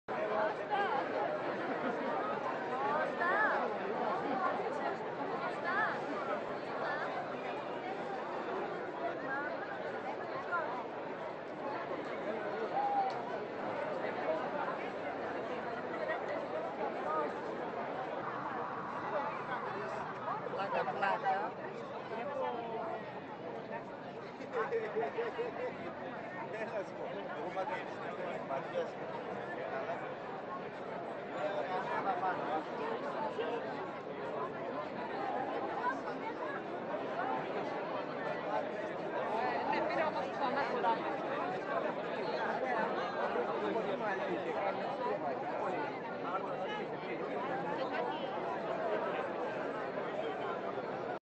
Ηράκλειο 25 Μαΐου 2011
Πλάνα από την Πλατεία Ελευθερίας την 25η Μαΐου 2011